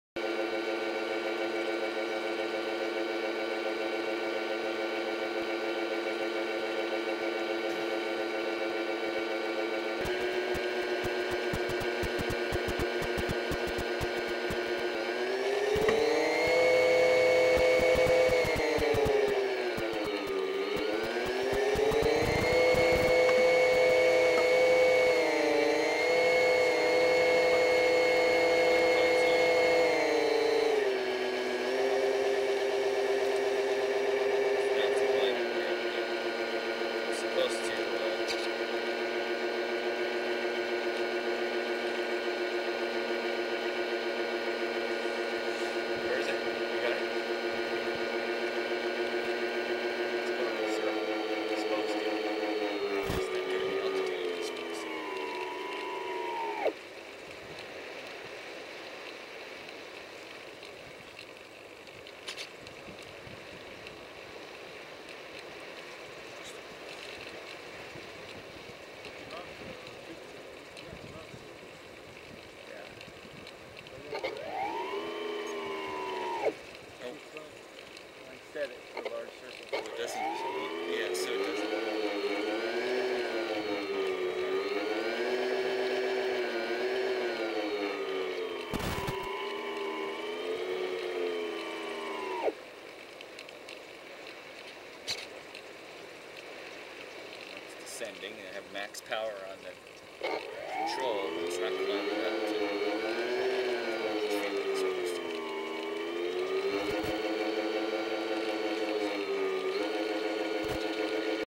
RTH throttle testing with airspeed sensor
DOSD RTH Throttle testing with airspeed sensor. I still cant get it to hold altitude.